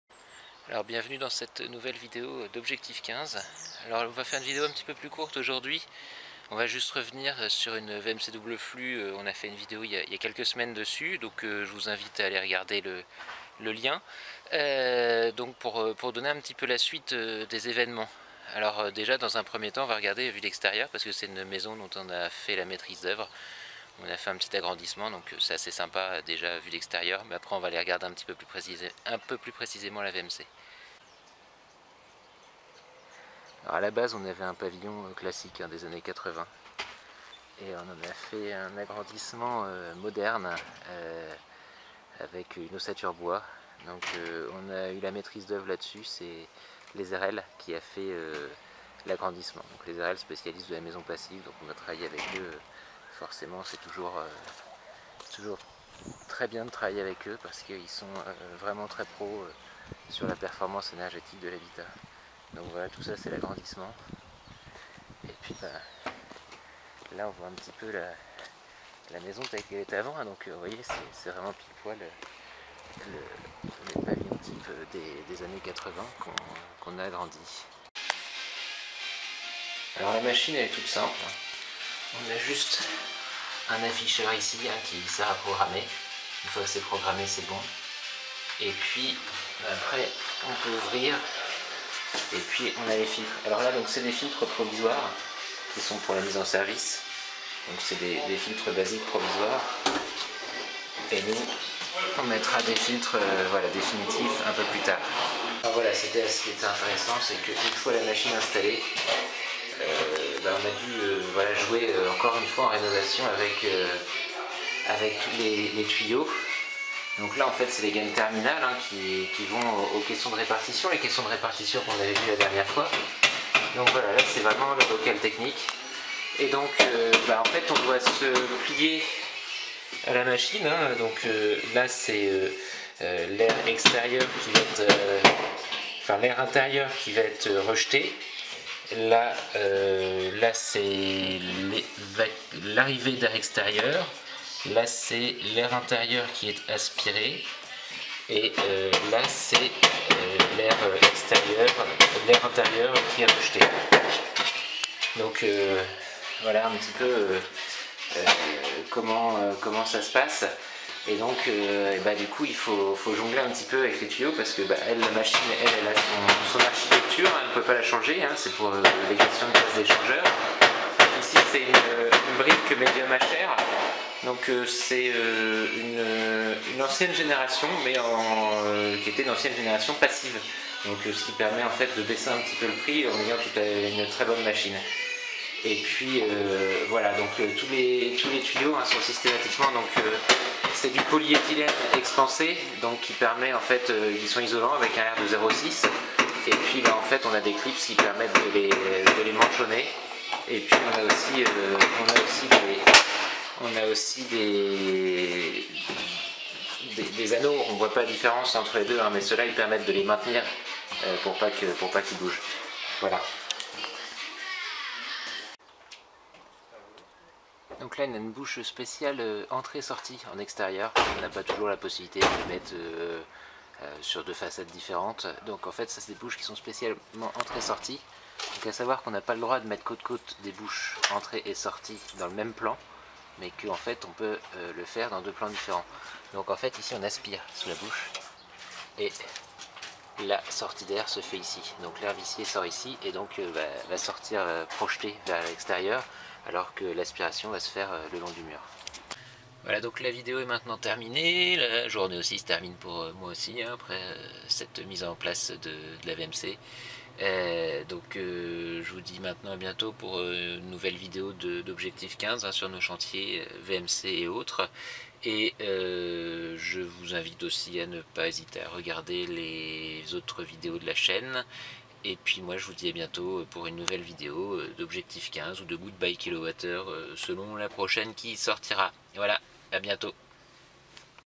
Agrandissement ossature bois et VMC double flux près de Rouen
La fin du chantier approche sur cette rénovation Objectif 15 ! Nous avons assuré la conception, la coordination et la VMC sur ce chantier. N'oublions pas Les airelles ) qui a réalisé une grande partie des travaux et notamment l'agrandissement en ossature bois.

Un chantier de rénovation de grande envergure et complexe réalisé dans un délai court.
Il est actuellement en fin de travaux, aussi, nous pouvons maintenant voir le résultat du travail, mais aussi les système entièrement installés.

Pour Objectif 15, c'est le cas de la VMC double flux Brink Medium HR qui a été posée fin février 2019.